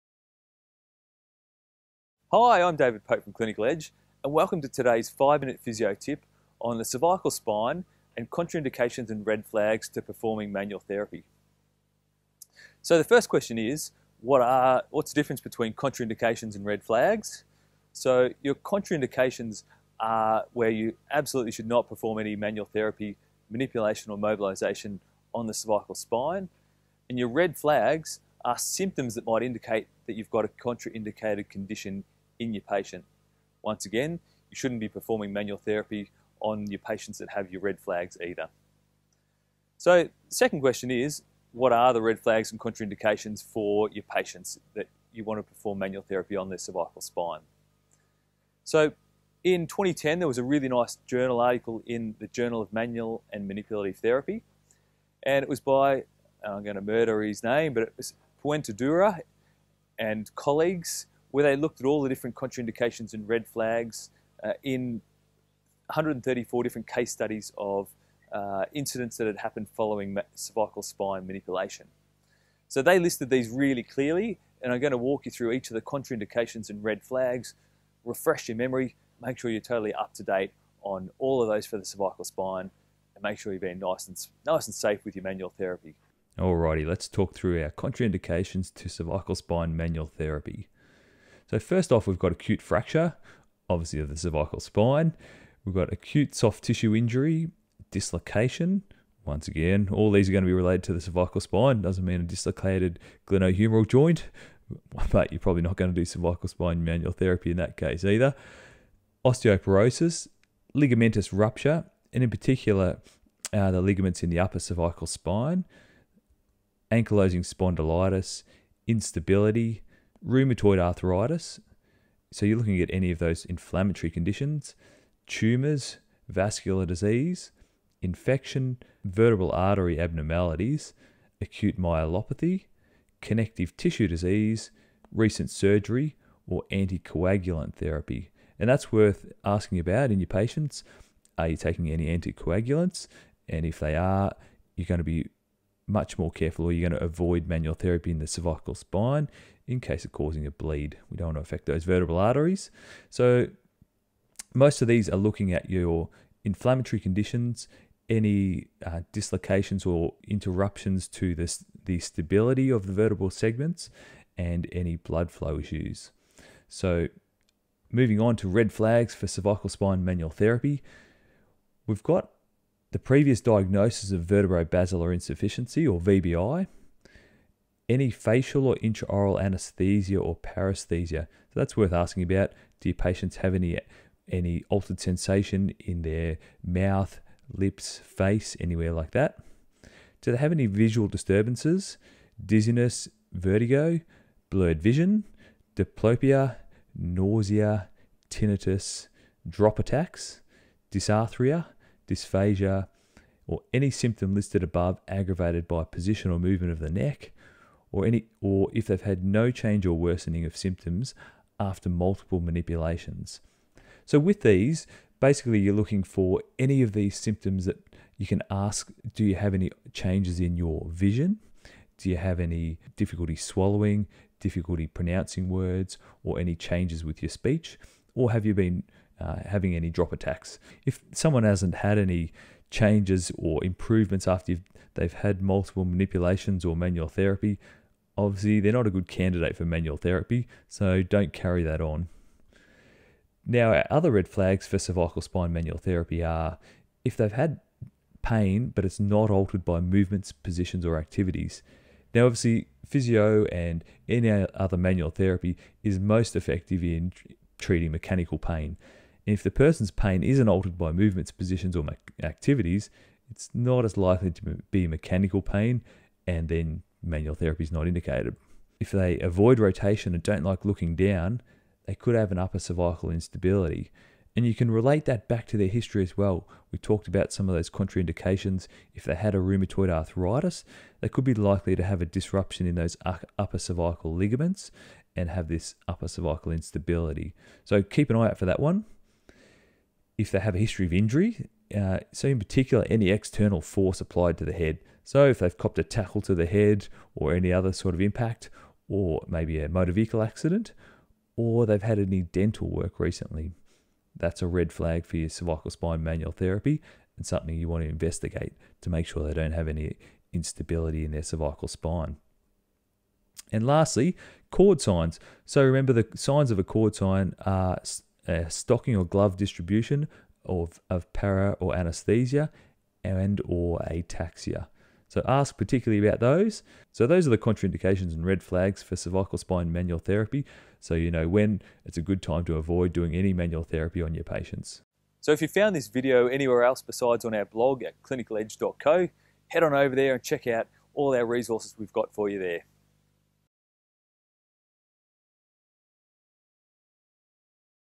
5 minute Physio tip - Contraindications and red flags to cervical spine manual therapy
What are the red flags and contraindications to cervical spine treatment? When should you avoid performing manual therapy on the cervical spine?

In this week's 5 minute Physio tip, I help refresh your memory on the red flags and contraindications to cervical spine manual therapy.